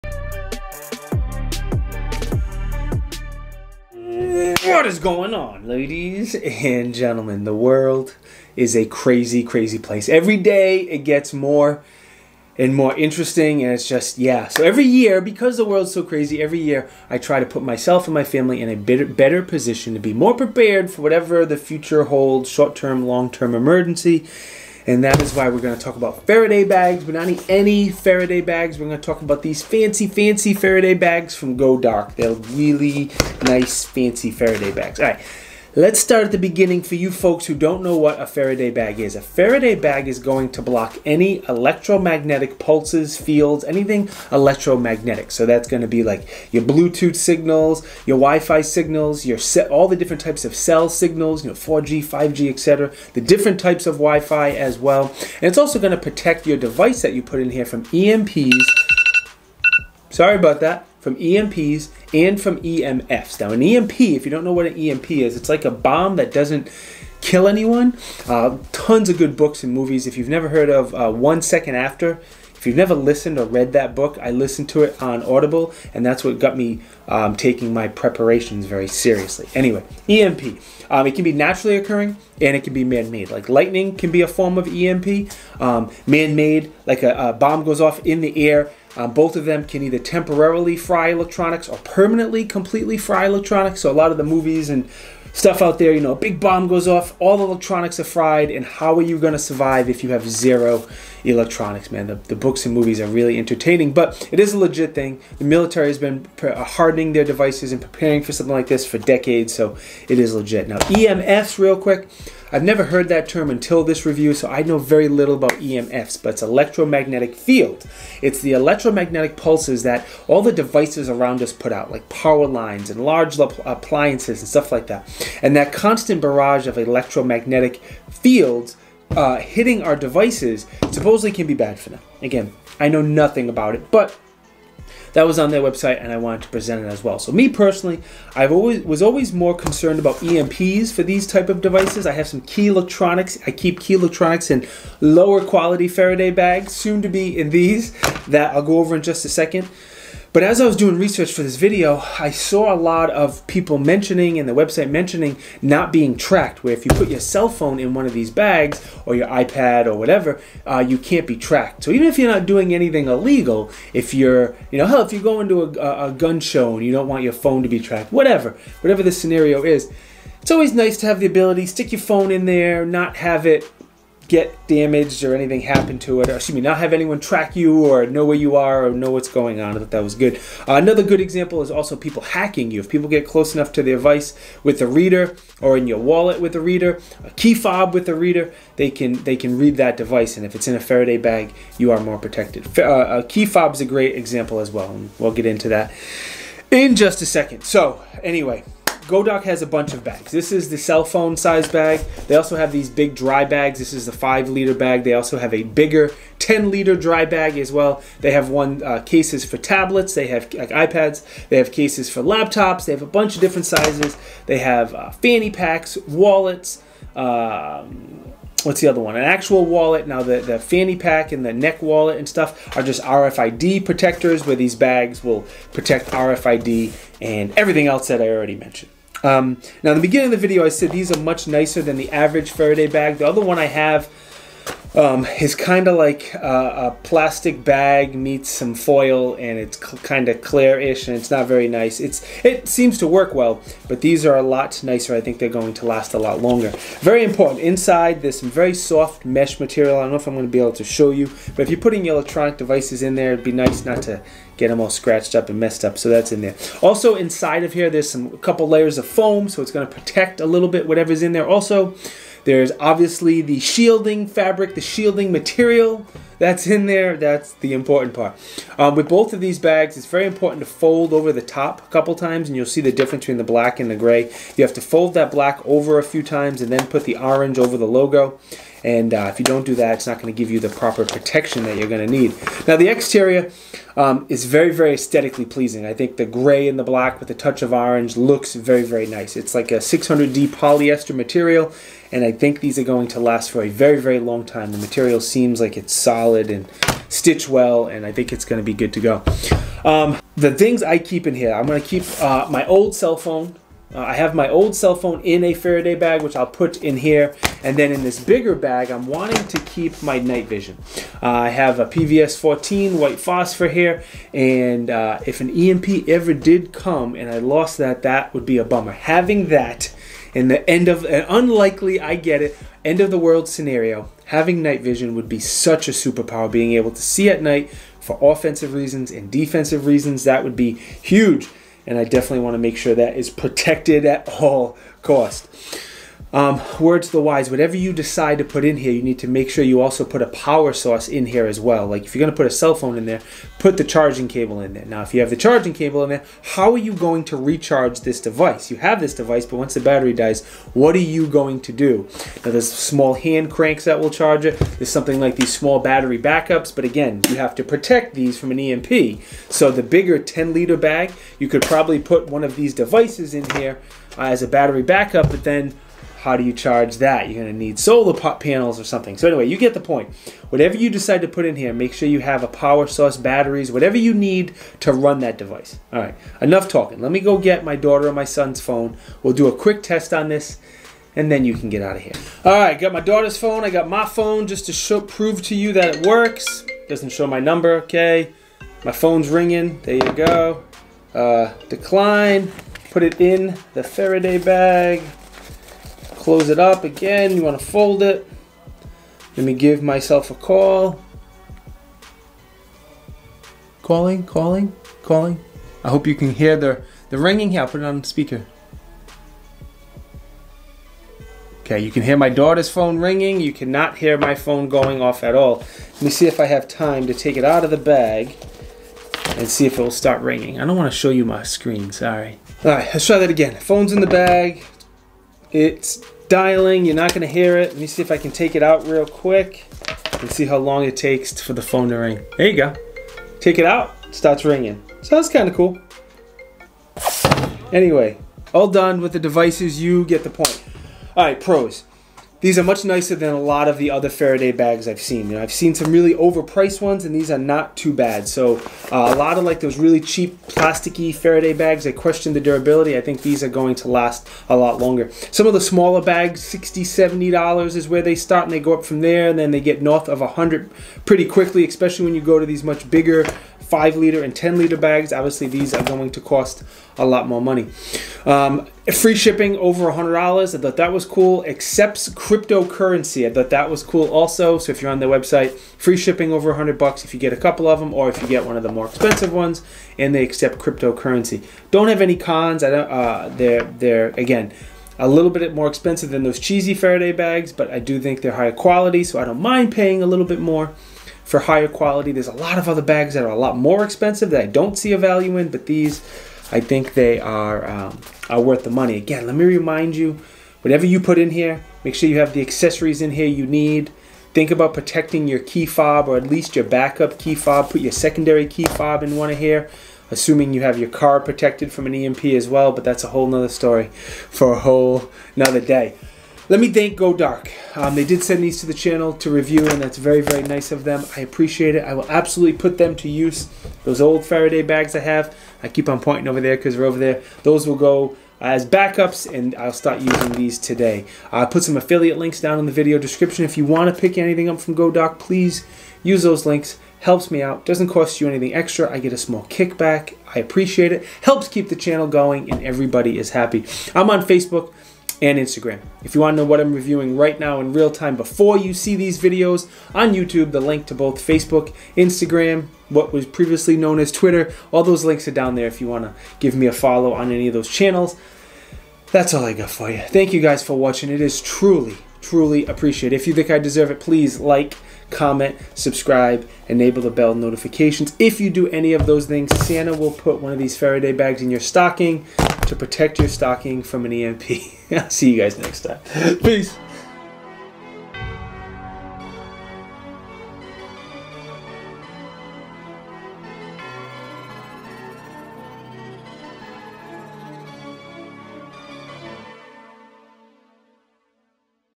GoDark Faraday Bag Review
The GoDark Faraday Bags, not only do these bags block EMP's and EMF's, they also block all bluetooth, wifi, gps signals, etc. Which means they are great at stopping you from being tracked, getting hacked, and protecting you from an EMP. These GoDark bags are better looking than most faraday bags and are available in several sizes including key fob, iPhone, iPad,  Laptop, and dry bags.

Tiberious Amazon Store